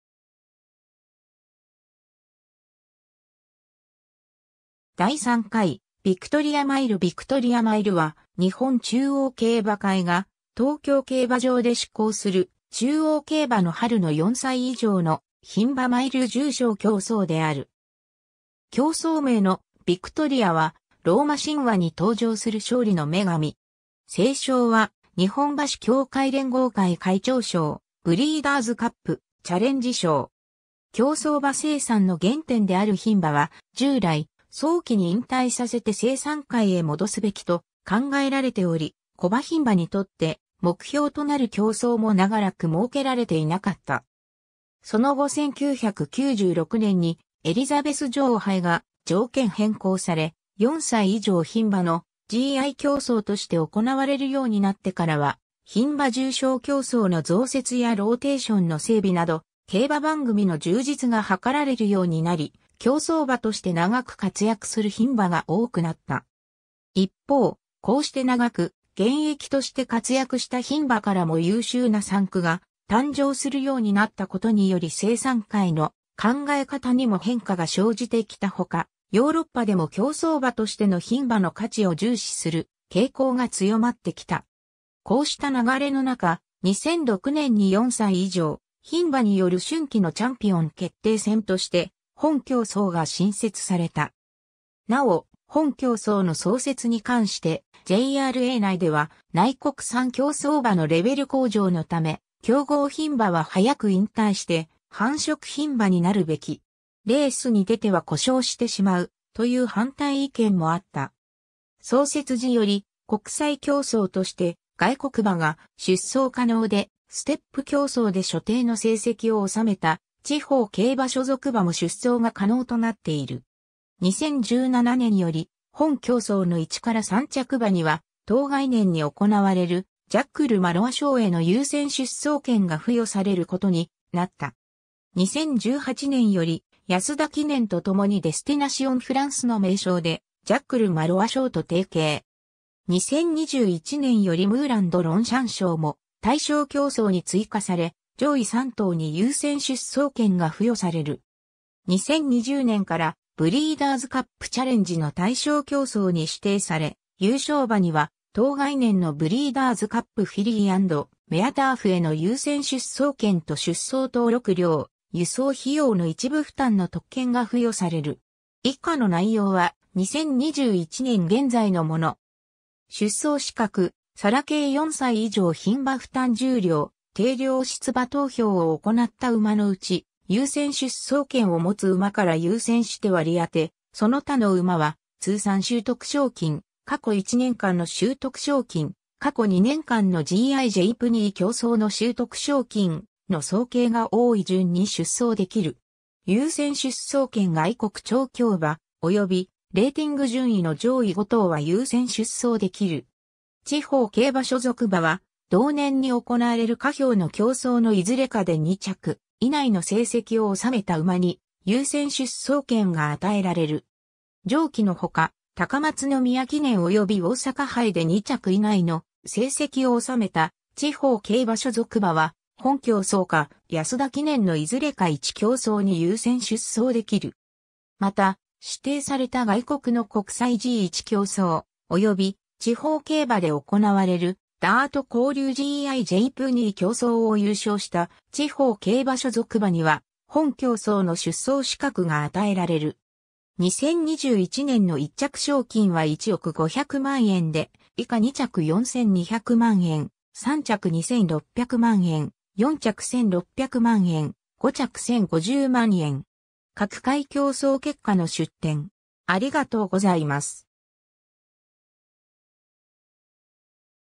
ヴィクトリアマイル
第3回ヴィクトリアマイル（2008年5月18日 エイジアンウインズ優勝） ヴィクトリアマイルは、日本中央競馬会（JRA）が東京競馬場で施行する中央競馬の春の4歳以上の牝馬マイル重賞競走（GI）である。
 競走名の「ヴィクトリア（Victoria）」は、ローマ神話に登場する勝利の女神。
 正賞は日本馬主協会連合会会長賞、ブリーダーズカップ・チャレンジ賞。
 競走馬生産の原点である牝馬は従来、早期に引退させて生産界へ戻すべきと考えられており、古馬牝馬にとって目標となる競走も長らく設けられていなかった。
その後1996年にエリザベス女王杯が条件変更され、4歳（現3歳）以上牝馬のGI競走として行われるようになってからは、牝馬重賞競走の増設やローテーションの整備など競馬番組の充実が図られるようになり、競走馬として長く活躍する牝馬が多くなった。
一方、こうして長く現役として活躍した牝馬からも優秀な産駒が誕生するようになったことにより生産界の考え方にも変化が生じてきたほか、ヨーロッパでも競走馬としての牝馬の価値を重視する傾向が強まってきた。
こうした流れの中、2006年に4歳以上牝馬による春季のチャンピオン決定戦として本競走が新設された。
なお、本競走の創設に関してJRA内では「内国産競走馬のレベル向上のため、強豪牝馬は早く引退して繁殖牝馬になるべき。レースに出ては故障してしまう」という反対意見もあった。
 創設時より国際競走として外国馬が出走可能で、ステップ競走で所定の成績を収めた地方競馬所属馬も出走が可能となっている。
 2017年より、本競走の1 - 3着馬には当該年に行われるジャック・ル・マロワ賞（G1）への優先出走権が付与されることになった。
2018年より安田記念とともにデスティナシオンフランスの名称でジャック・ル・マロワ賞と提携。
2021年よりムーラン・ド・ロンシャン賞（G1）も対象競走に追加され、上位3頭に優先出走権が付与される。
 2020年からブリーダーズカップ・チャレンジの対象競走に指定され、優勝馬には当該年のブリーダーズカップ・フィリー&メアターフへの優先出走権と出走登録料・輸送費用の一部負担の特権が付与される。
 以下の内容は、2021年現在のもの。
 出走資格：サラ系4歳以上牝馬 負担重量：定量（55kg） 出馬投票を行った馬のうち、優先出走権（次節参照）をもつ馬から優先して割り当て、その他の馬は「通算収得賞金」+「過去1年間の収得賞金」+「過去2年間のGI・JpnI競走の収得賞金」の総計が多い順に出走できる。
 優先出走権 外国調教馬、およびレーティング順位の上位5頭（2012年より。106ポンド以上であることが条件）は優先出走できる。
 地方競馬所属馬は同年に行われる下表の競走のいずれかで2着以内の成績を収めた馬に、優先出走権が与えられる。
 上記のほか、高松宮記念及び大阪杯で2着以内の成績を収めた地方競馬所属馬は本競走か安田記念のいずれか1競走に優先出走できる。
また、指定された外国の国際G1競走（2歳G1競走は除く）、および地方競馬で行われるダート交流GI・JpnI競走（2歳GI・JpnI競走は除く）を優勝した地方競馬所属馬には本競走の出走資格が与えられる。
 2021年の1着賞金は1億500万円で、以下2着4200万円、3着2600万円、4着1600万円、5着1050万円。
 各回競走結果の出典